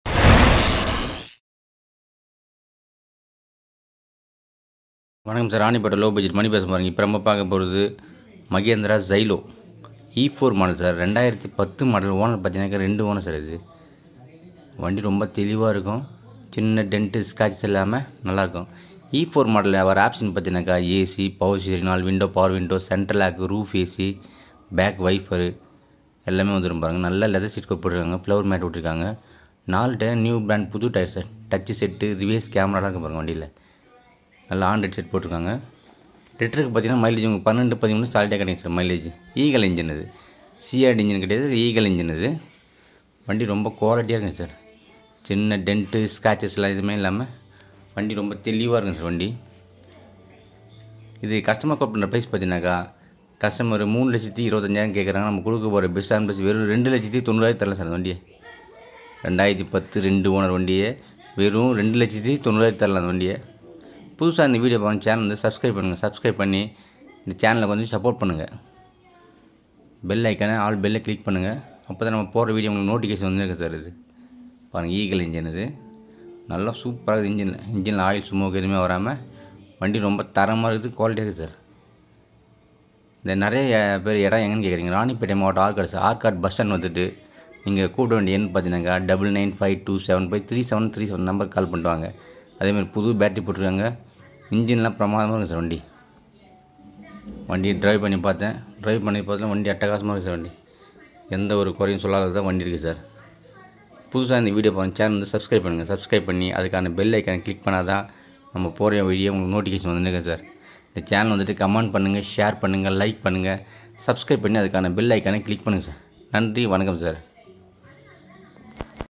LOW BUDGET MANI CARS ARCOT
MAHINDRA XYLO E4 MODEL OWNER SECOND, AC POWER STEERING POWER WINDOW, ROOF AC, BACK VAIFER, REVIEWS CEMARA, CENTRE LOCK, DIESEL,NEW BRAND TAYAR SHEET COVER NEW INSURANCE CURRENT VERY GOOD CONDITION, CONTACT NUMBER ,